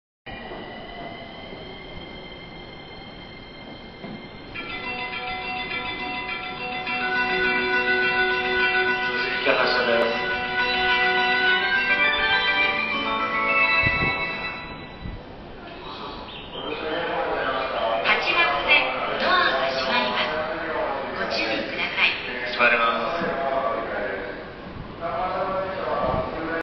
JR東日本 大船駅 8番線発車メロディー
JR東日本大船駅8番線の発車メロディーです。